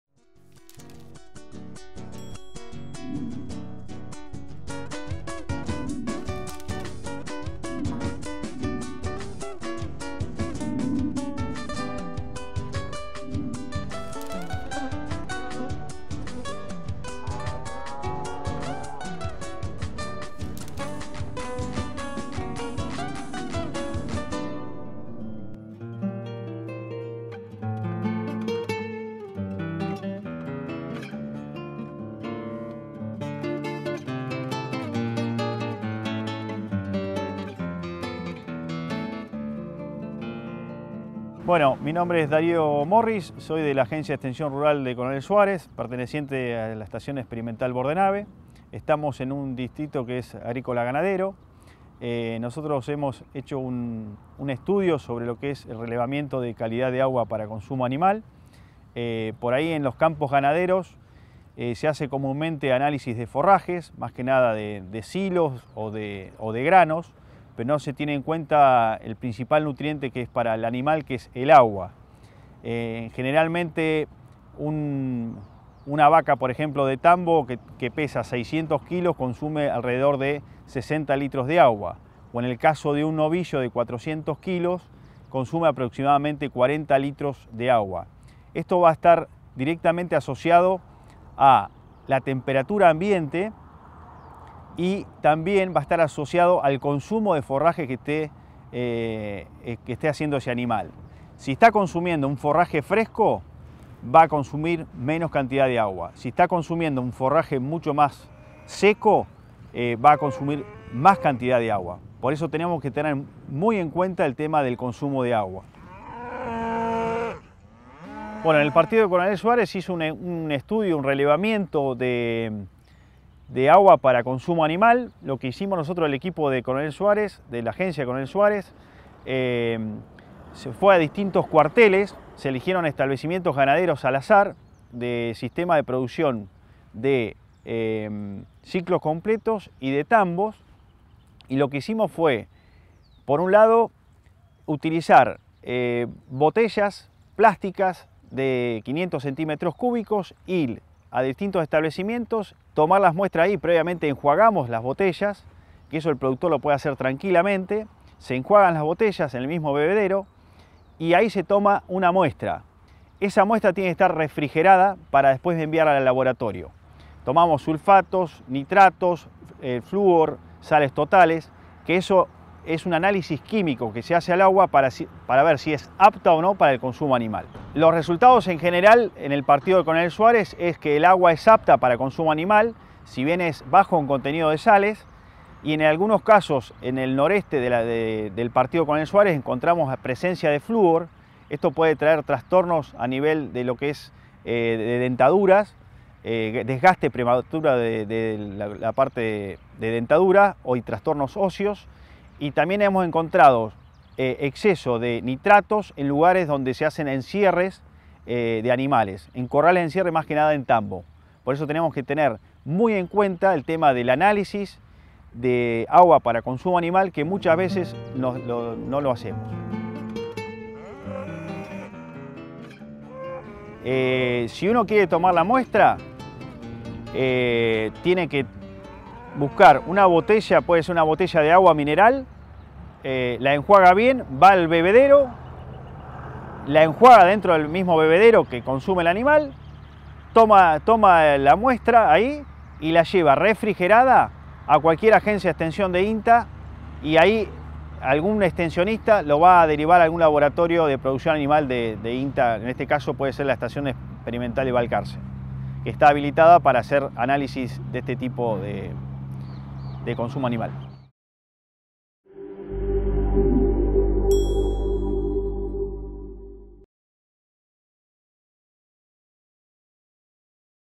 Calidad de agua para consumo animal
En los sistemas ganaderos se realizan diversos análisis (calidad de forraje, alimentos concentrados, etc.), pero muchas veces no se toma en cuenta la calidad del agua para el consumo animal dentro del establecimiento. Y esto es un grave error, ya que el agua es el nutriente más importante en la producción animal, porque se utiliza para la nutrición y el crecimiento.

En la nota, el Ing. Darío Morris se refiere a aspectos claves del tema: la importancia de que el animal disponga de cantidad y calidad suficiente de agua -ya que va a influir en otros factores, como el consumo de alimento y su productividad-; aspectos que determinan el consumo de agua, como la temperatura ambiente y el consumo de materia seca; diferencias de consumo según factores biológicos (raza, edad, estado fisiológico), ambientales (temperatura, humedad, etc.) y otros aspectos relacionados con la dieta.

También menciona el relevamiento realizado en el partido de Coronel Suárez por técnicos de la Agencia de Extensión Rural local, perteneciente a INTA Bordenave, e indica el modo correcto de tomar una muestra para enviar a analizar a un laboratorio.